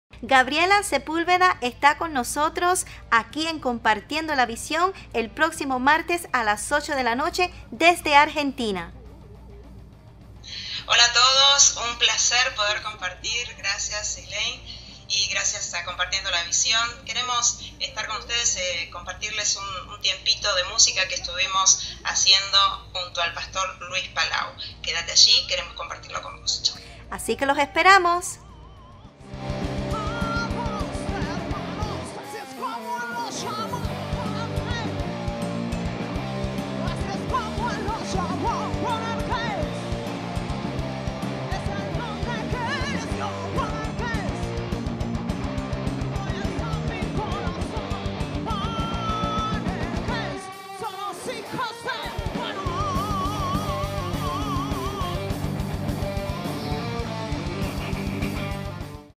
Próximo Martes 4 de Diciembre en Compartiendo La Visión - Gabriela Sepúlveda
Saludos amigos! Los esperamos el próximo Martes 4 de Diciembre a las 8 pm, en el Programa CompartiendoLaVisión con Elaine Enriquez por OVMRadio. Junto a nuestra invitada, Gabriela Sepúlveda. No te lo pierdas! Escuchanos por ww.OVMRadio.com